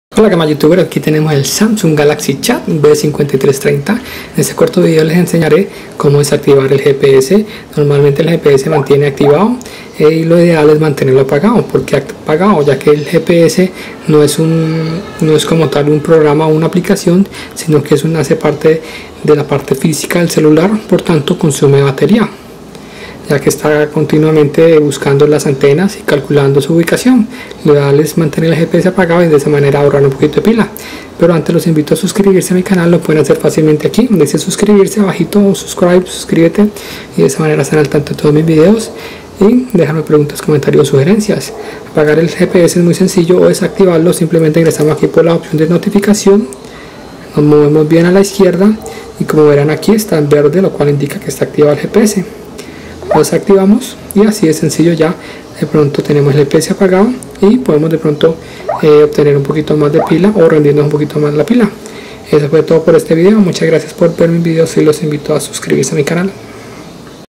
como desactivar el gps del samsung Galaxy chat B5330 español Full HD
B5330 Cómo apagar el GPS de tu samsung Galaxy chat español YouTube